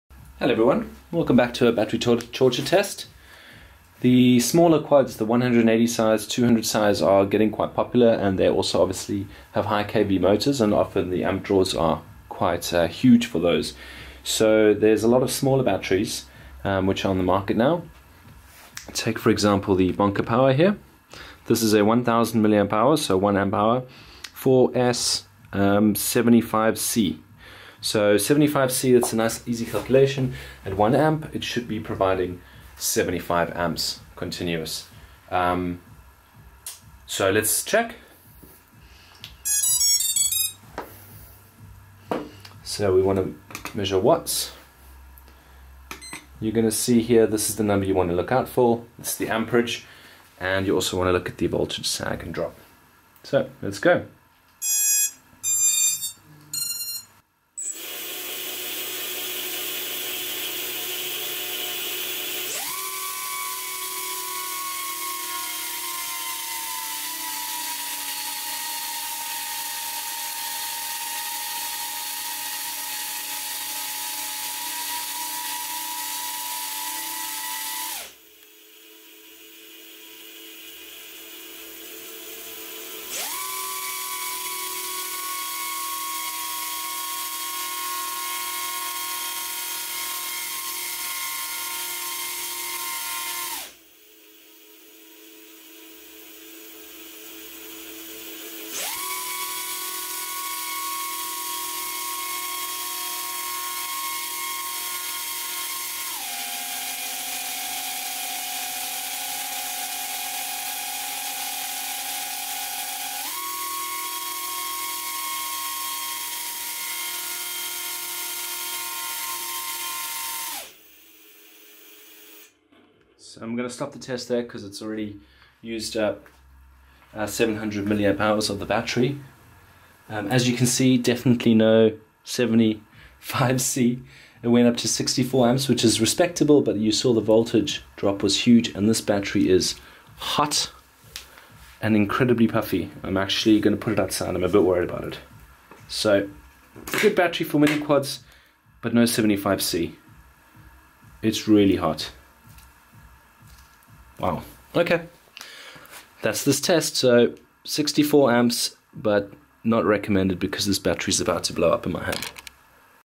Battery Torture Test Bonka 75C 1000
The BTT for the Bonka 70C 1000mAh miniquad battery